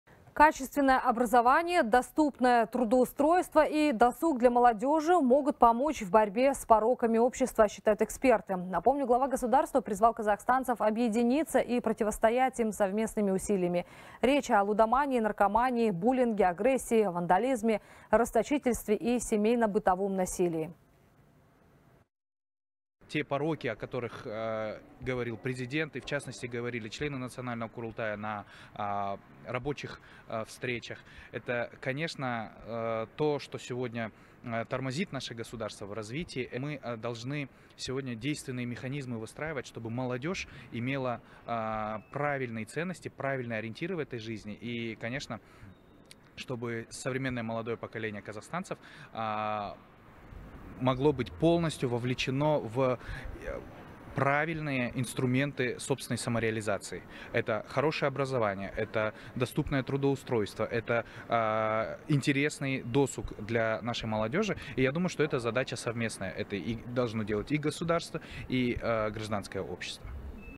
Что поможет в борьбе с пороками общества
Качественное образование, доступное трудоустройство и досуг для молодого поколения могут помочь в борьбе с пороками общества, считают эксперты, передаёт «24KZ».